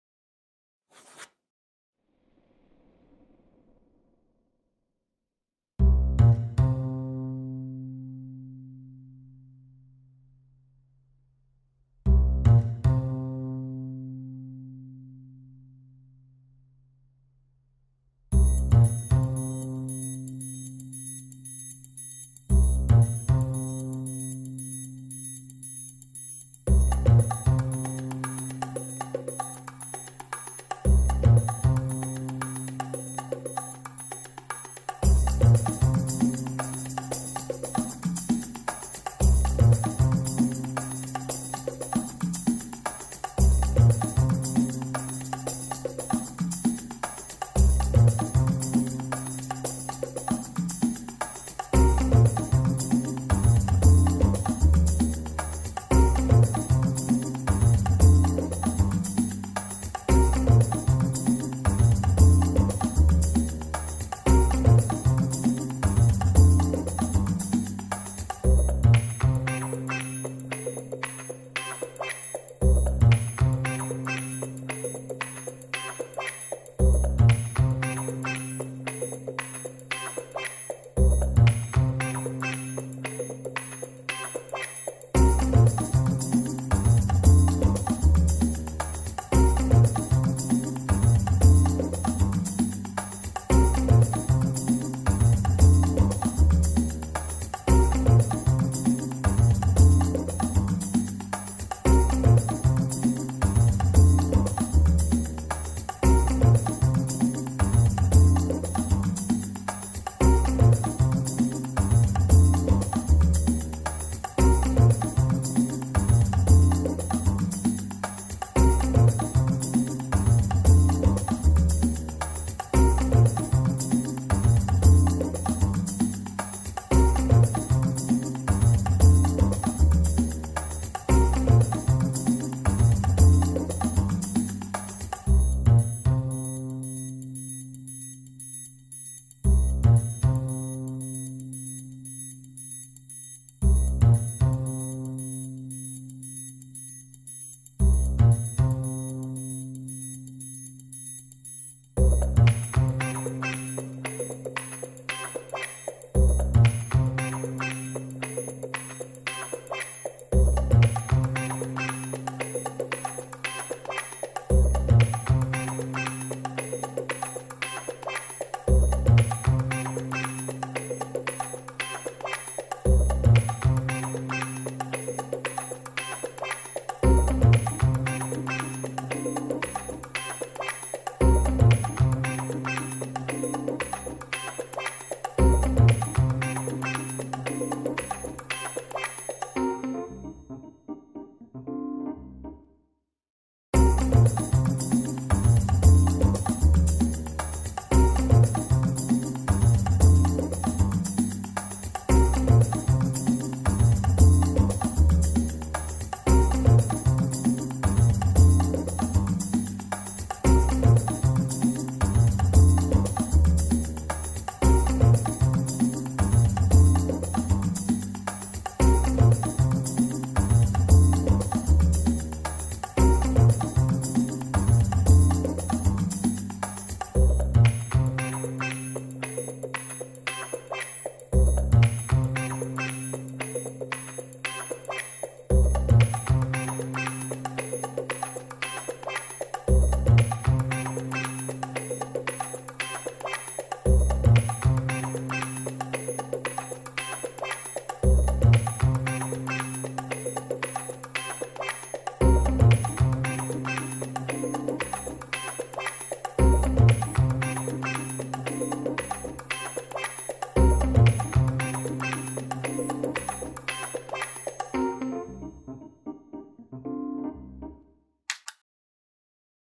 2020 Sucks, a Timelapse
Had some technical and scheduling troubles over the last year, but I'm finally uploading the timelapse of a painting I did last September, and taking the opportunity to freshen up the presentation on the videos a little.

Missed out the bit where I put the logo on her shirt, so if you just wanted to see that, maybe you should skip this one :)

Reference photograph by Sabrina Kennelly, pexels.com (adjusted)